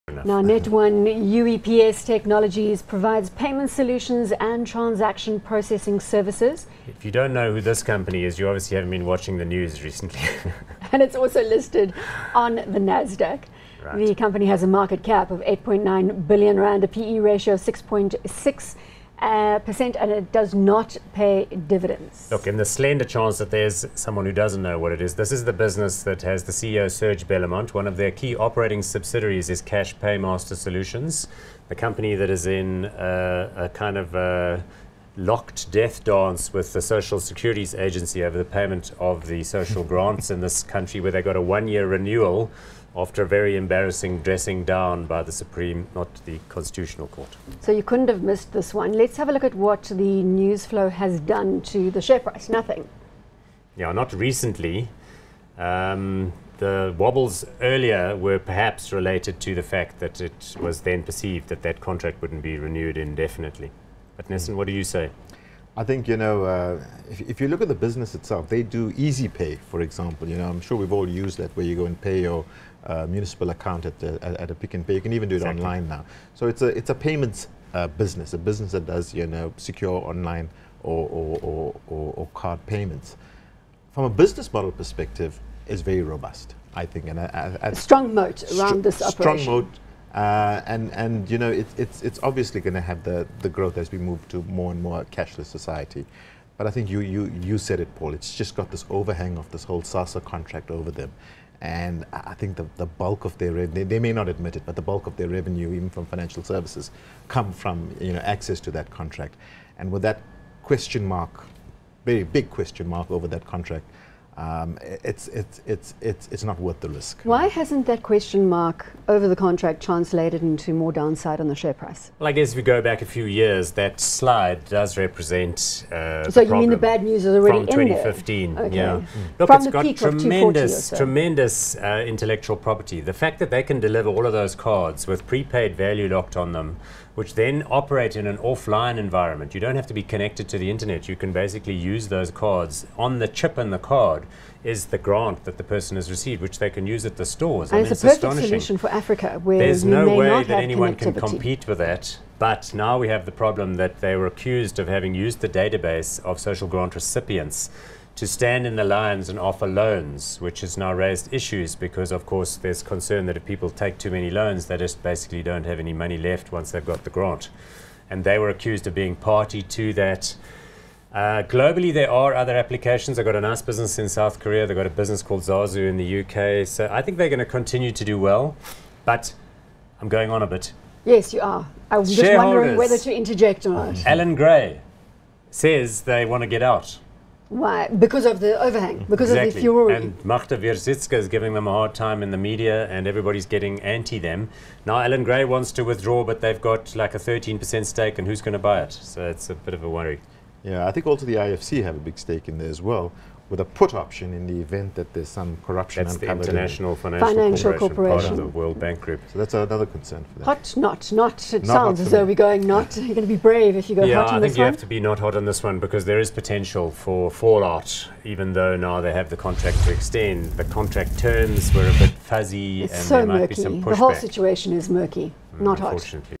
Net 1-UEPS - Hot or Not
Net 1-UEPS Technologies provides payment solutions and transaction processing services. It is also listed on the Nasdaq. The company has a market cap of R8.9 billion and a PE ratio of 6.61. It does not pay dividends.Guest Nesan Nair, Stockbroker & Portfolio Manager Sasfin Securities and resident expert Nitrogen Fund Managers from Vestact decide whether Net 1-UEPS is hot or not.